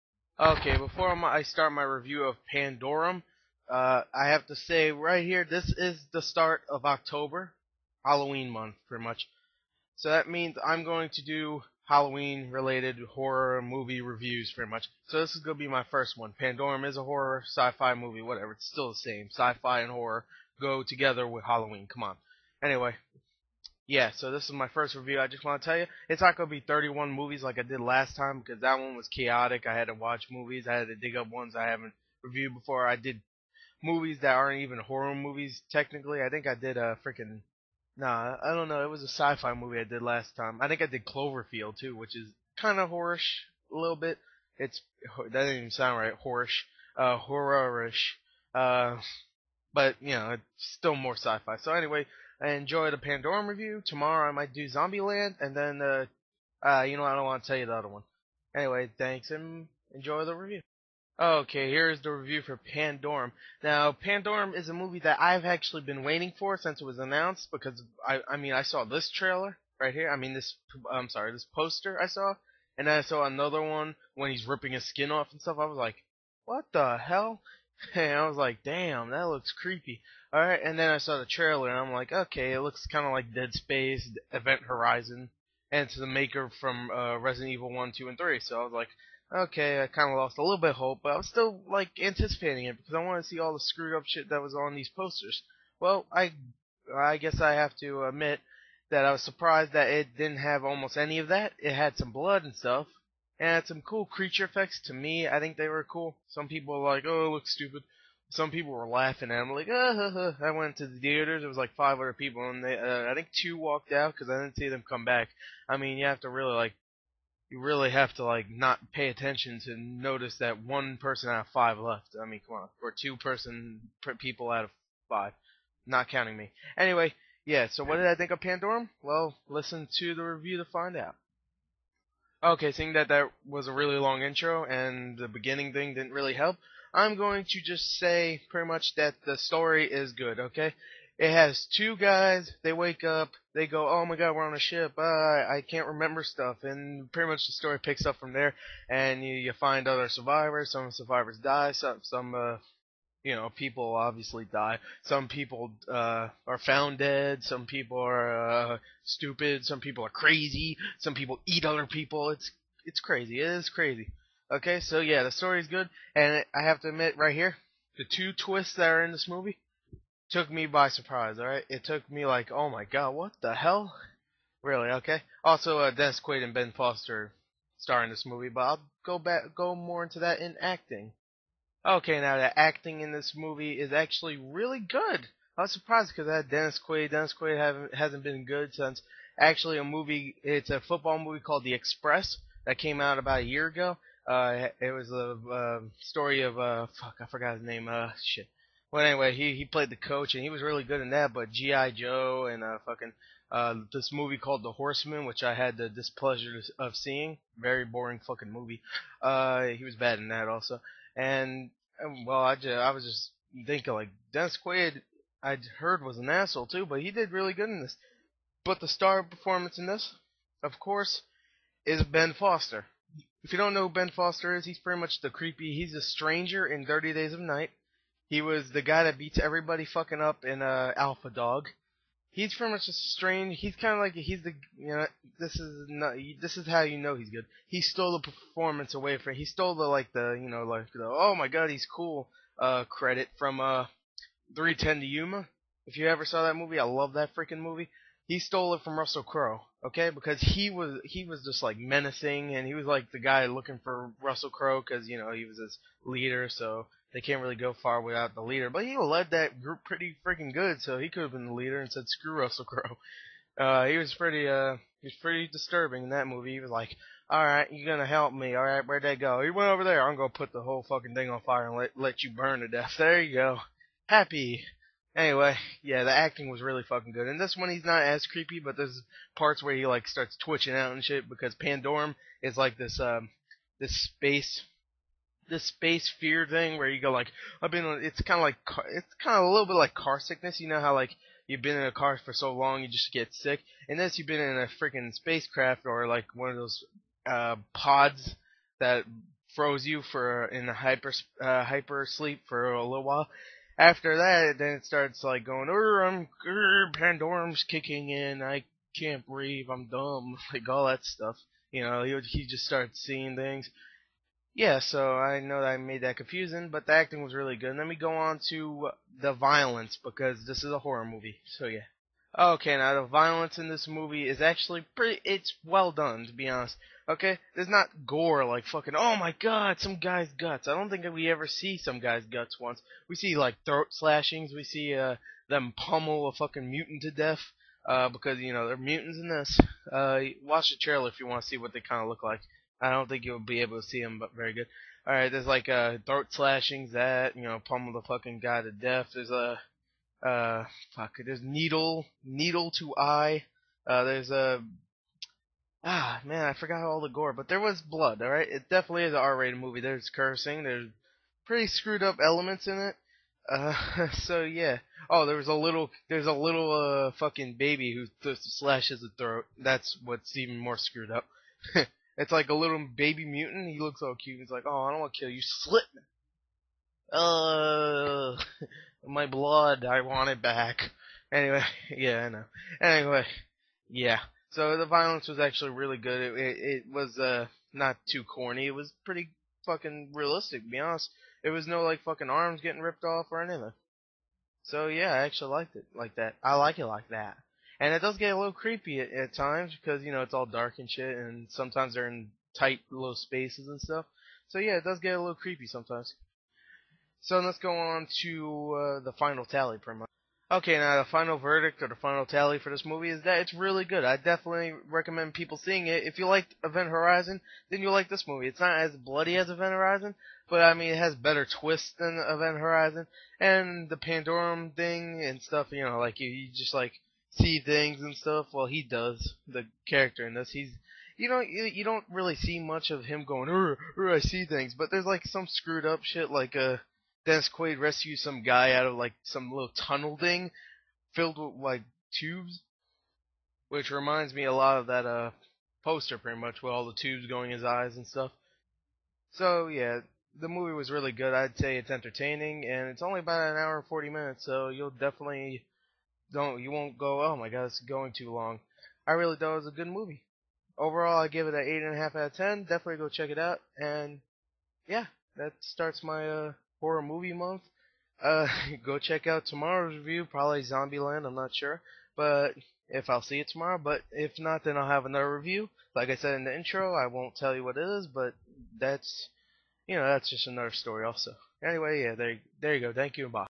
Pandorum - Review
Gors Horror Movie Month #1

My review of the much anticipated (at least for me) horror sci fi movie. Liking the videos makes sure you see my videos more on the front page! You love me...right?